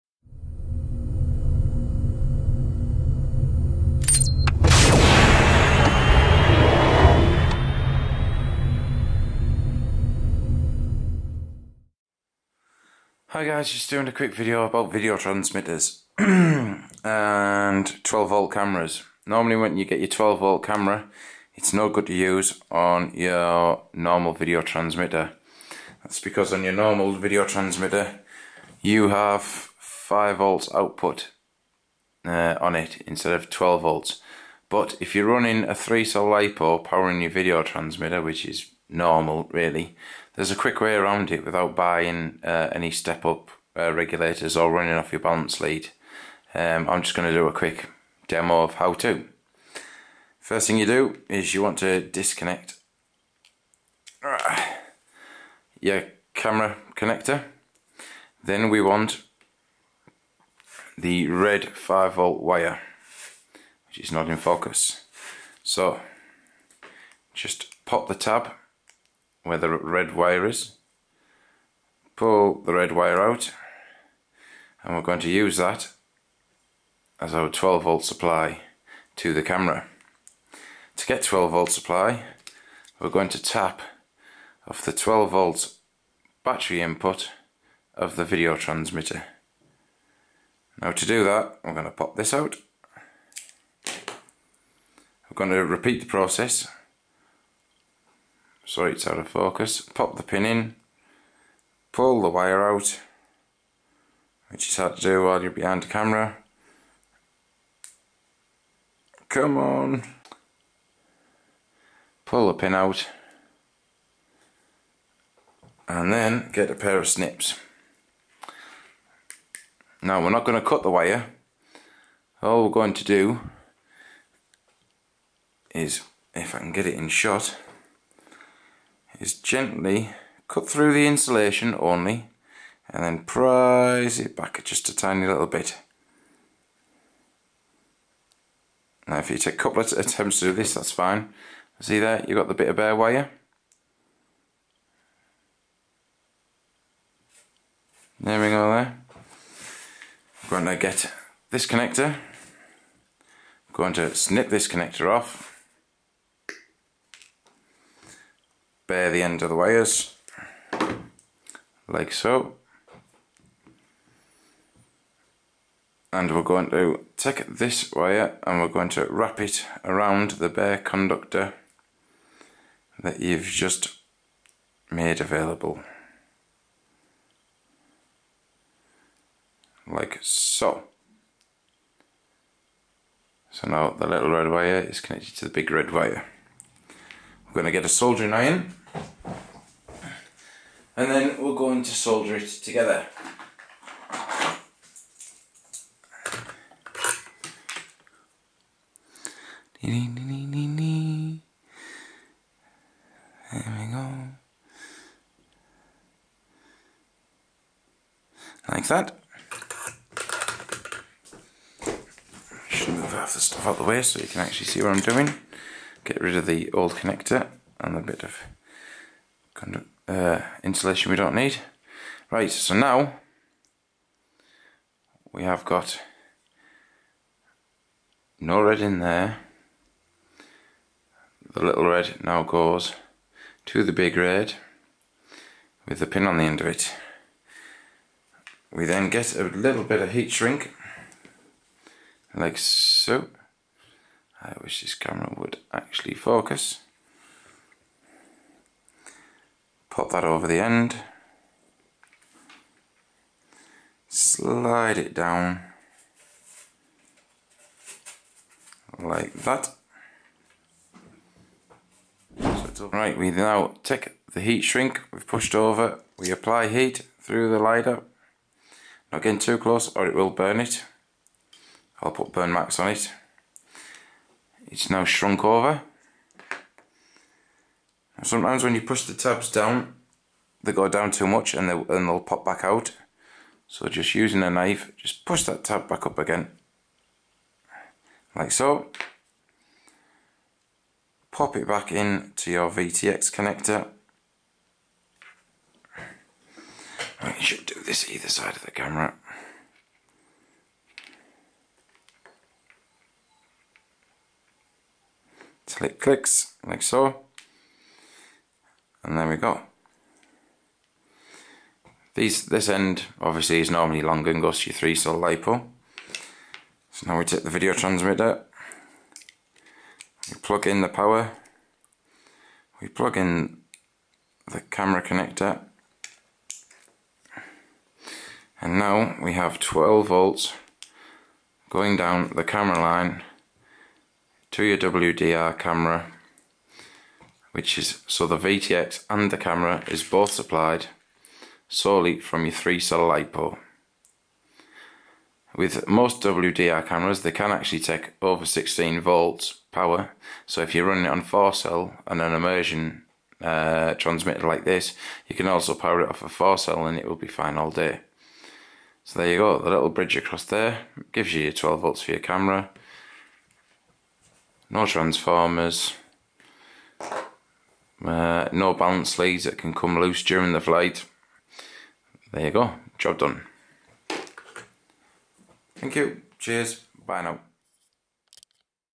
12v Camera Hack
A quick guide to hacking an ImmersionRC Vtx to power a WDR camera ( 12v )